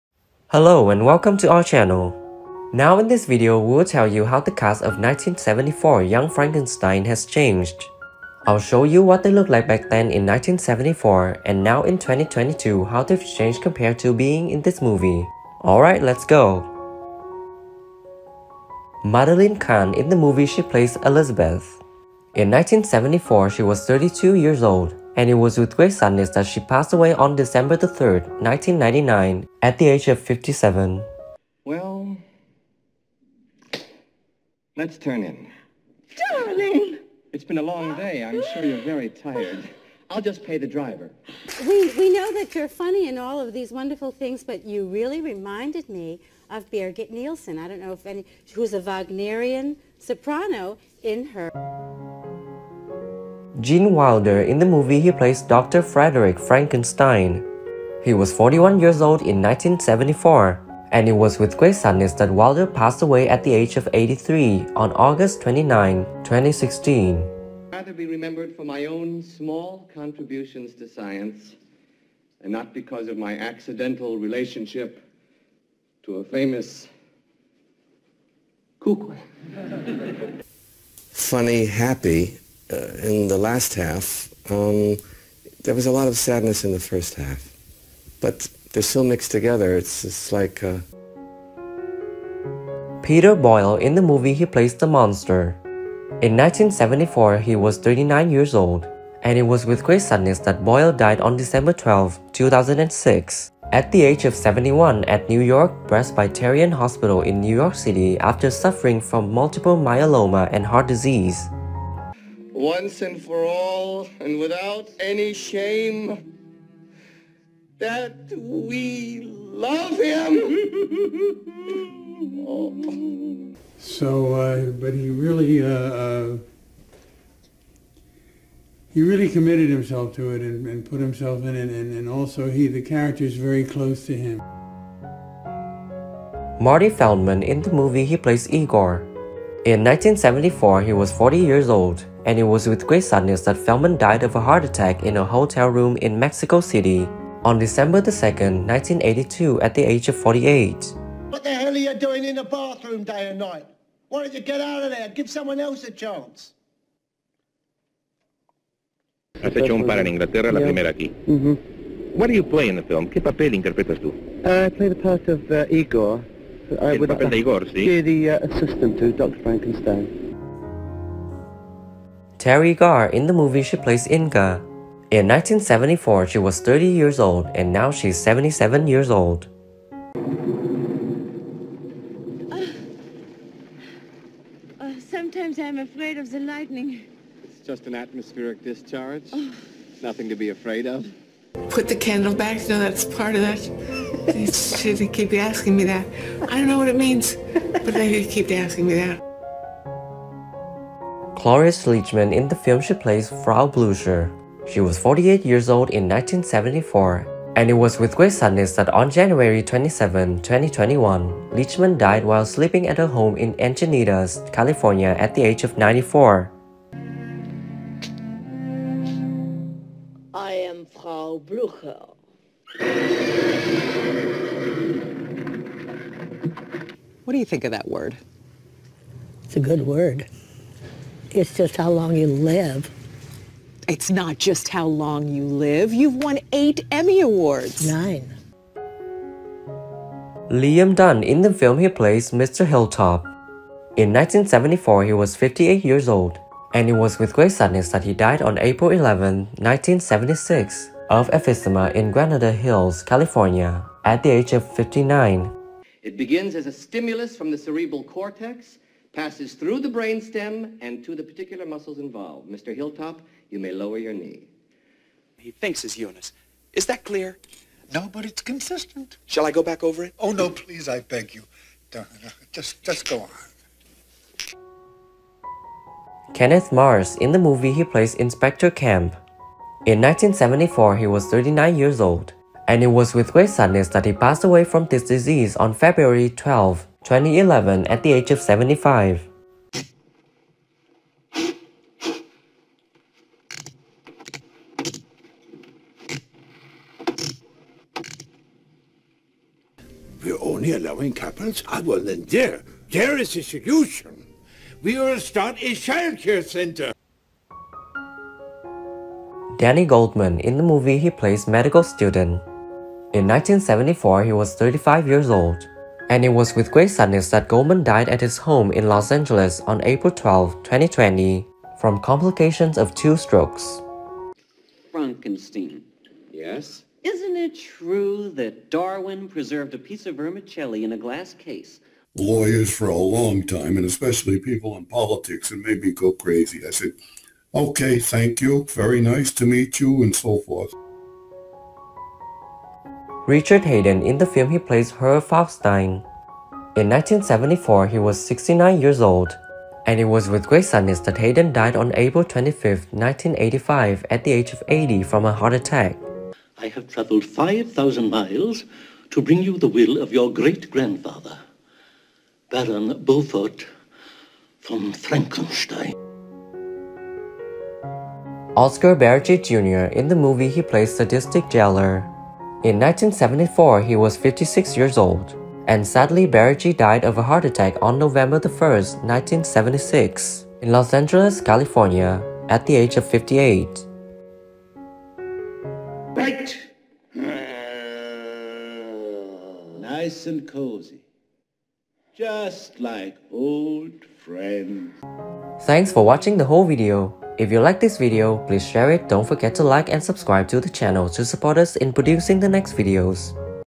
Young Frankenstein 1974 Cast: THEN and NOW [48 Years After]
Young Frankenstein is a 1974 American comedy horror film directed by Mel Brooks. The screenplay was co-written by Brooks and Gene Wilder. Wilder also starred in the lead role as the title character, a descendant of the infamous Dr. Victor Frankenstein. Peter Boyle portrayed the monster.[4] The film co-stars Teri Garr, Cloris Leachman, Marty Feldman, Madeline Kahn, Kenneth Mars, Richard Haydn, and Gene Hackman.
_Starring:
Gene Wilder
Peter Boyle
Marty Feldman
Cloris Leachman
Teri Garr
Kenneth Mars
Madeline Kahn